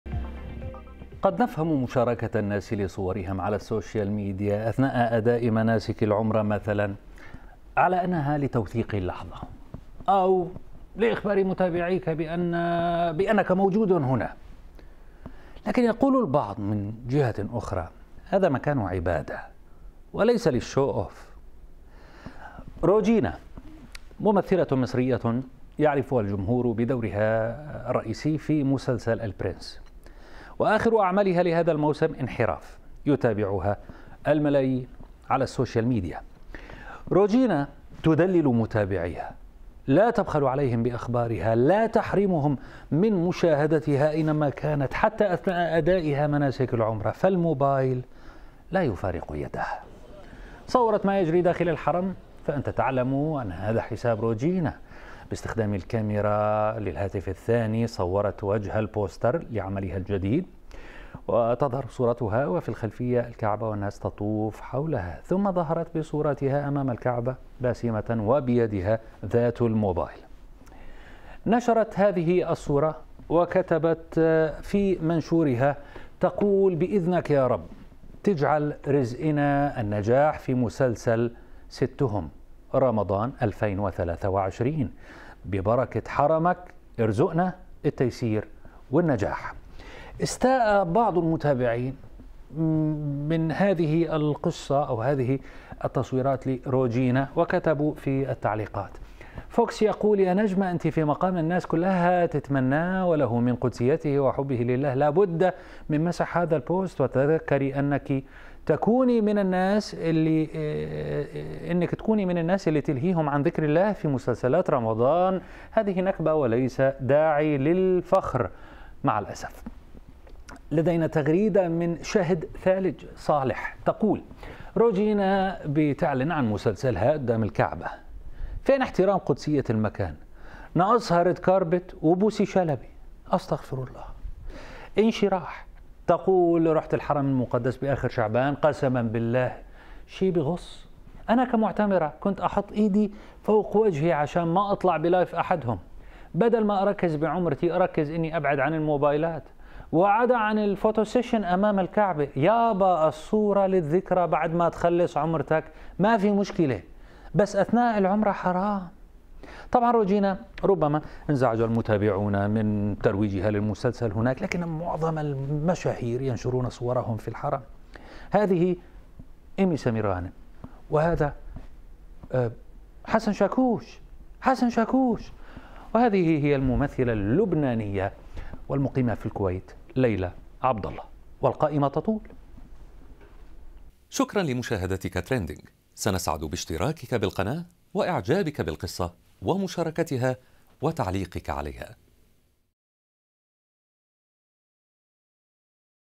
صورة للفنانة المصرية روجينا أمام الحرم المكي تثير انتقادات
صورة للفنانة روجينا أمام الحرم المكي تثير الغضب
 ثار غضب متابعي الممثلة المصرية روجينا على وسائل التواصل الاجتماعي، إثر نشرها صورة لترويج مسلسلها الجديد من أمام الحرم في مكة المكرمة